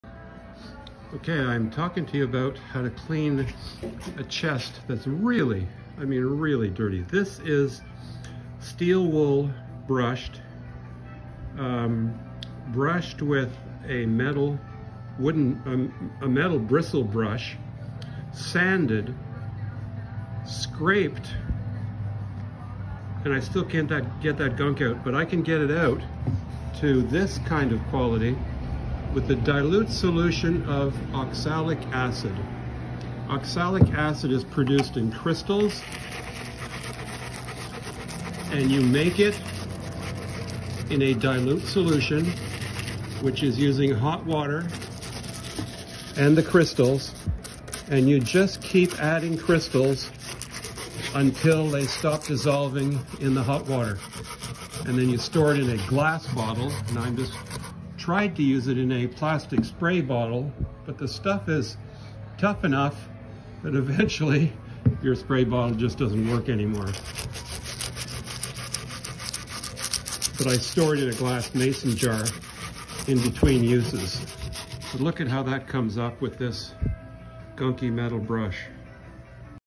How to clean an antique wood chest
I’m working with a rather old chest that has been in storage and neglected for decades and the filth has penetrated deep into the wood. Someone tore off the canvas on several sides now the challenge is to restore the wood. This is the first of a few videos on this process